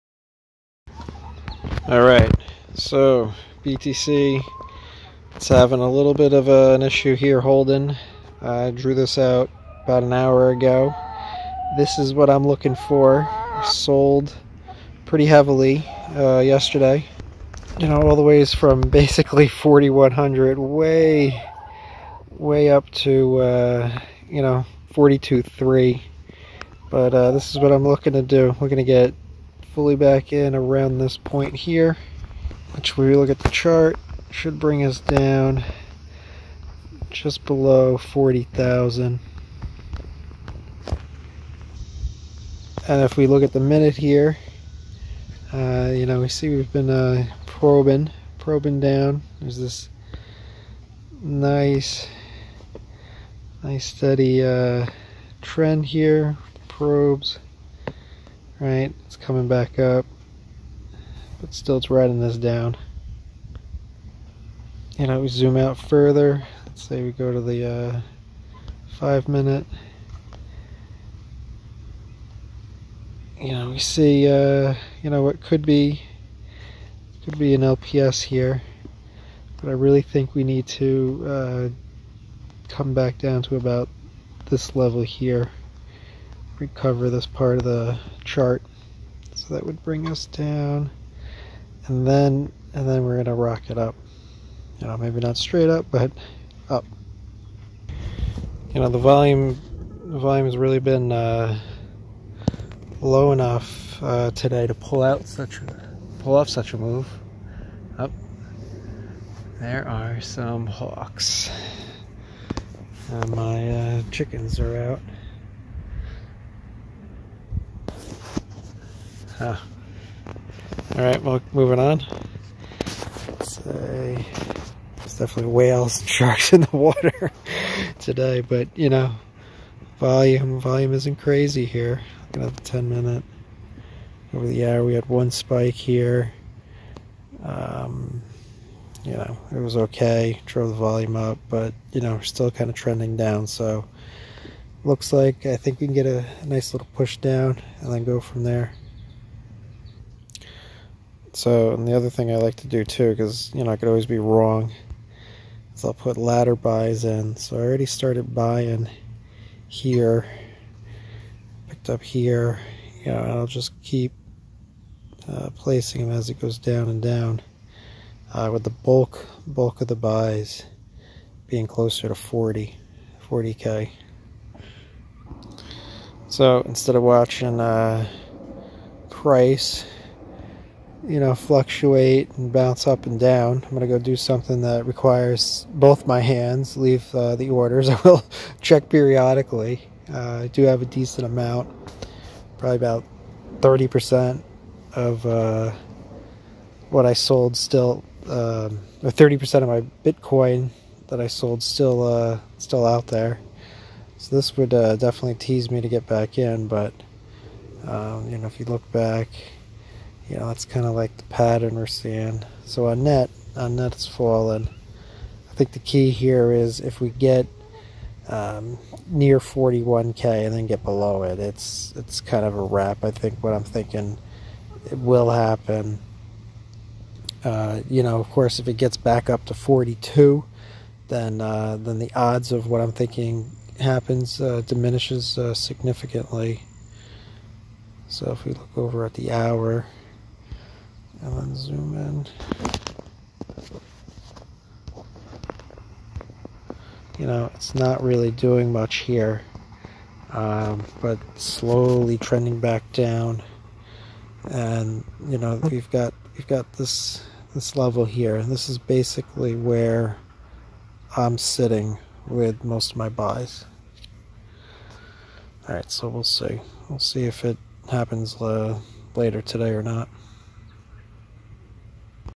July 31st - Is Bitcoin Falling? Target 40k Before 🚀
Had a great meal and sold some BTC last night above 42k.  Waiting now to get back in around 40k before the big rally.